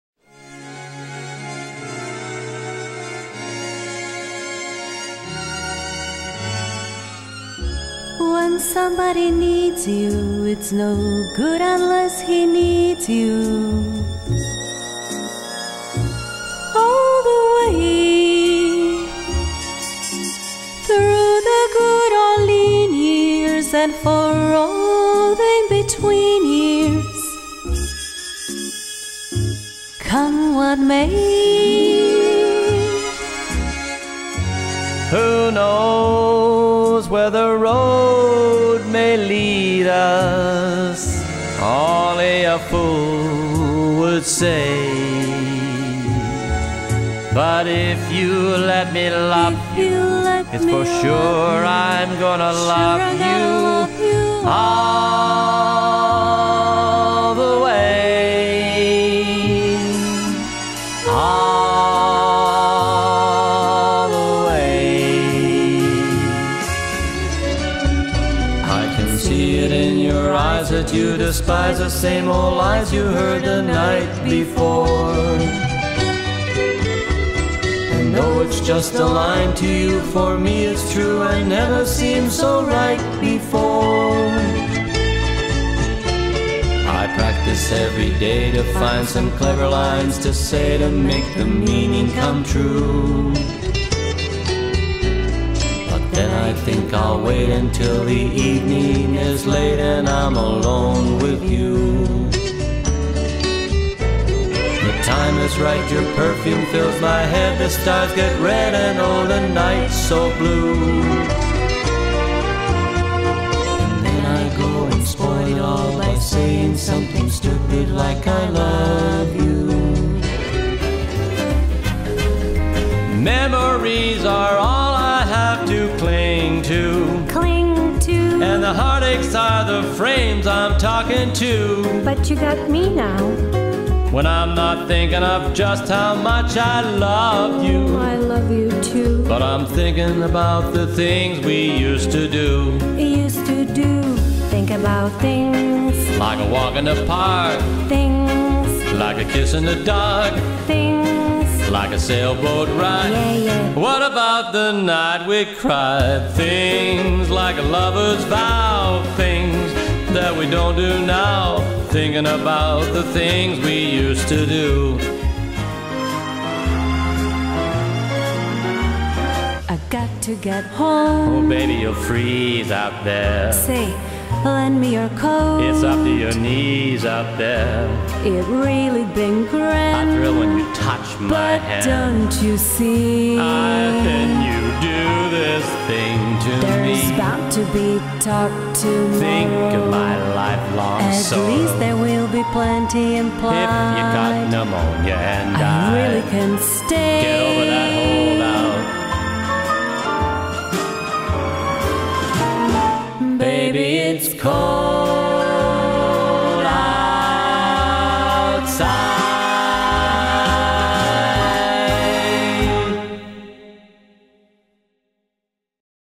RICK AND MIA, GOLDEN CLASSICS / DUETS (cover)
Rick Valiant of SONGS OF SINATRA, tribute and MIA of THE GOLDEN CLASSICS join their acts.